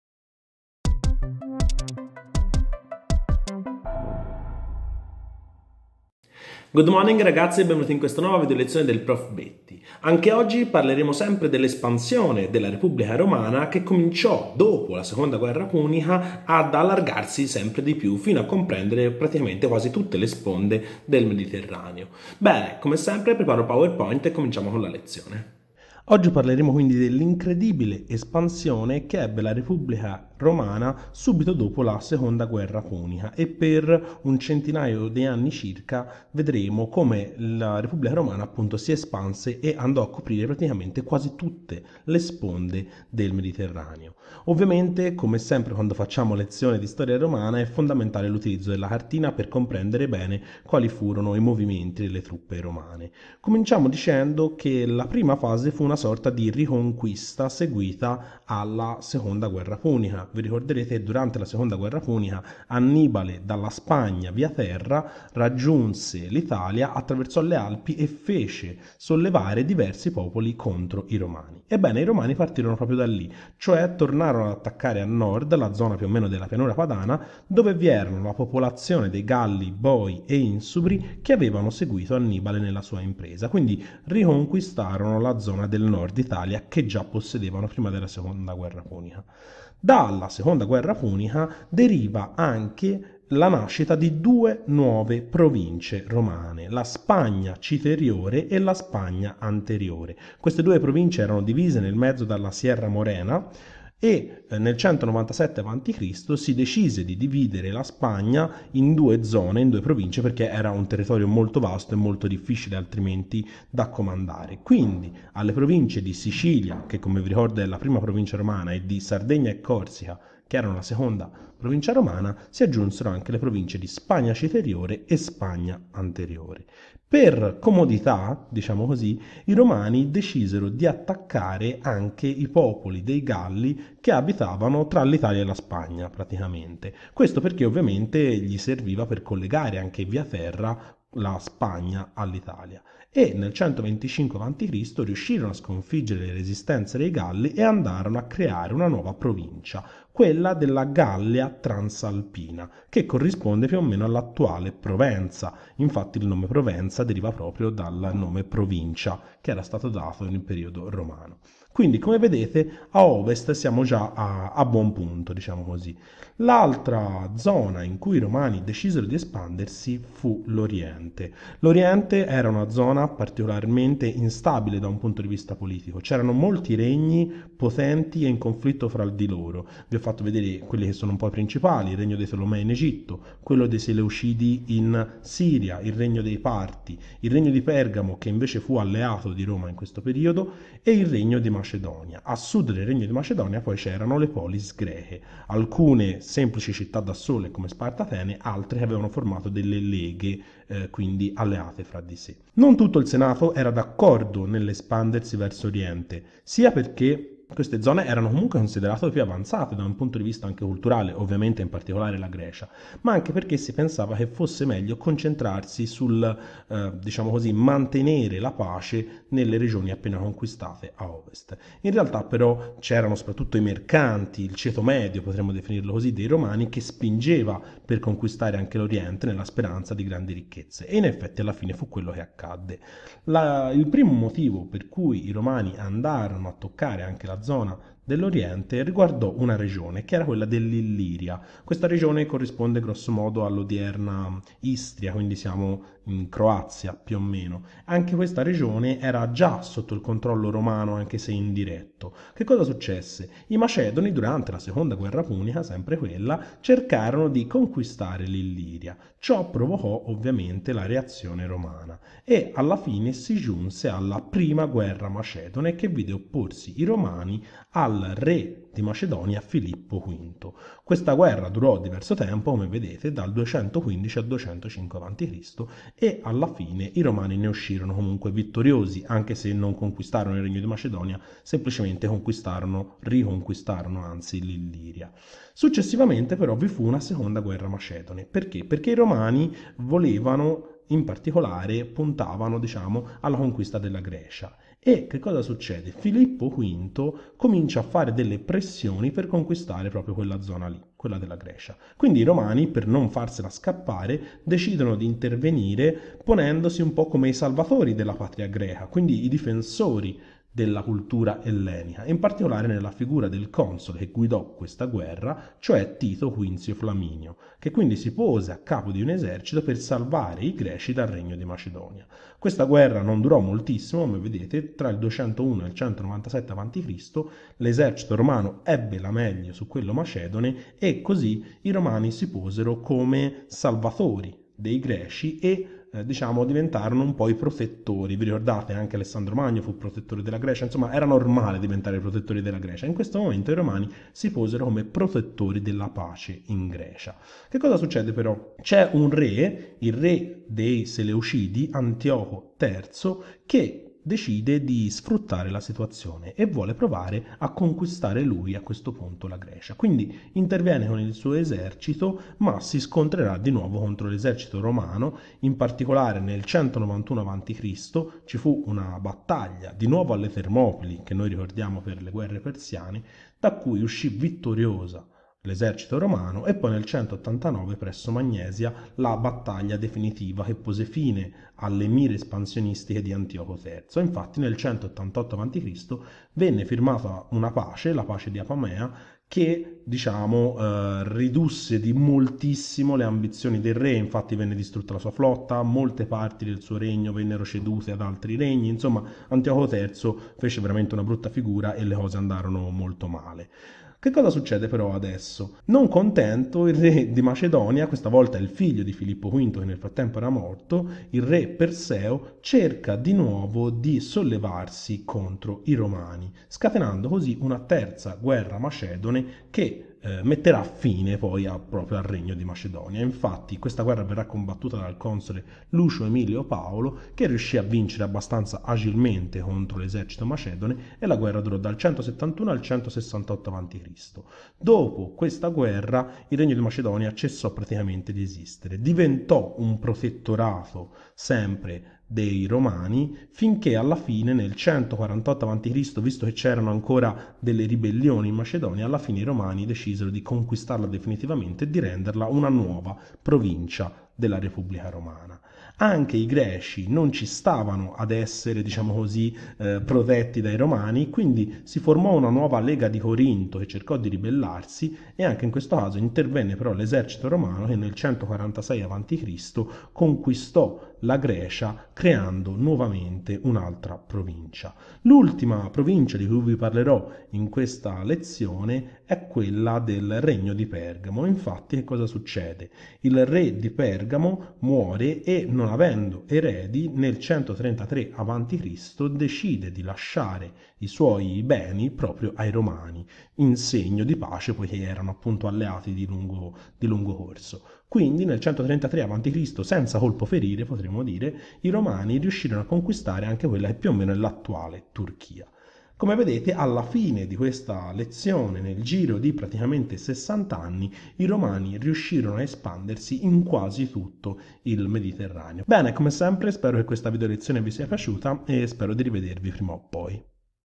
L'espansione romana nel Mediterraneo - Prof Betti
In questa videolezione vedremo la potenza romana che continua ad espandersi verso ovest (Spagna e Gallia Transalpina) e verso est (guerre macedoni e egemonia sulla Grecia).